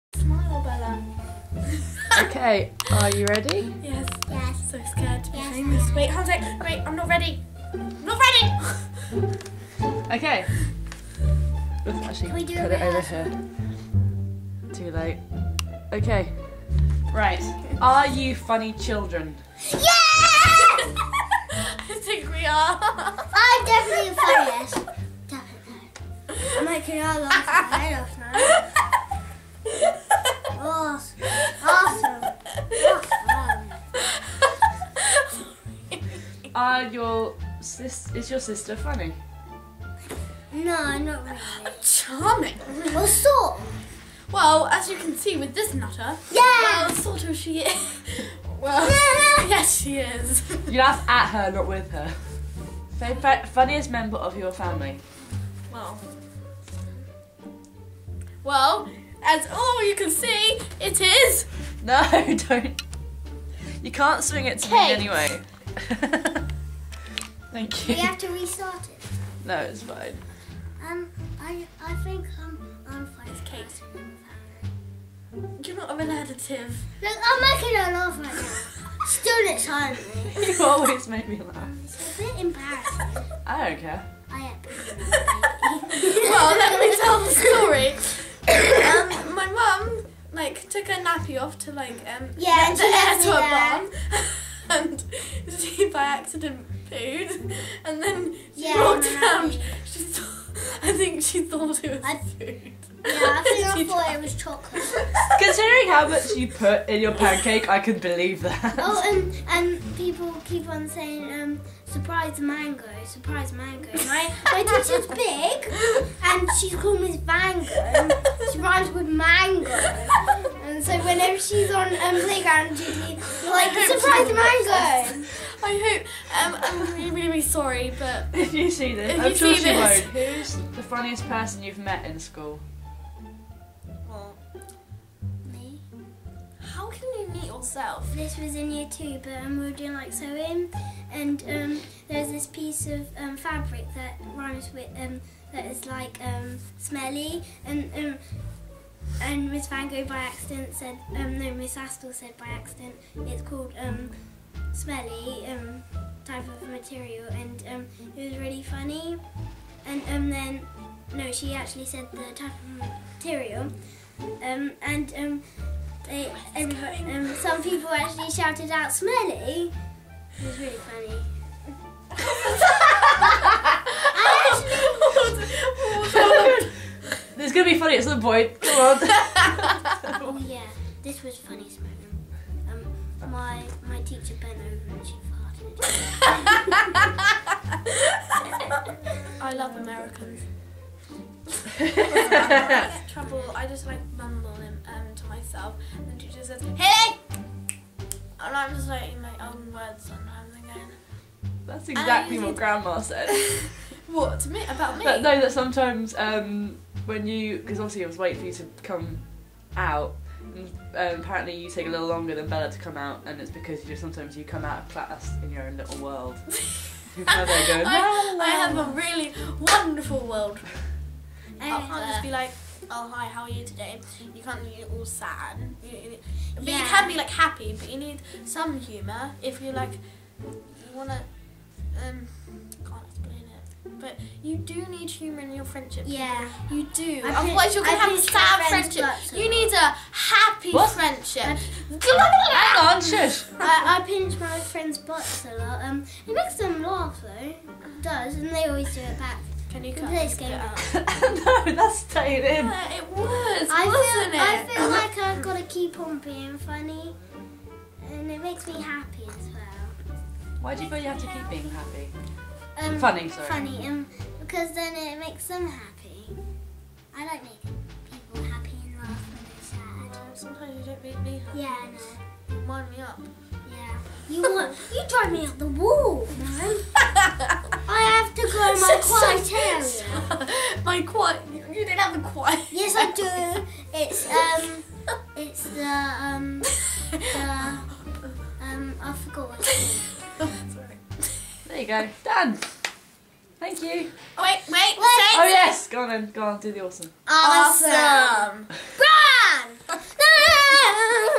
Laughter; A Cousin-To-Cousin Interview
A short film involving me interviewing my two young cousins about humor and how its a part of their lives. WARNING: Very adorable.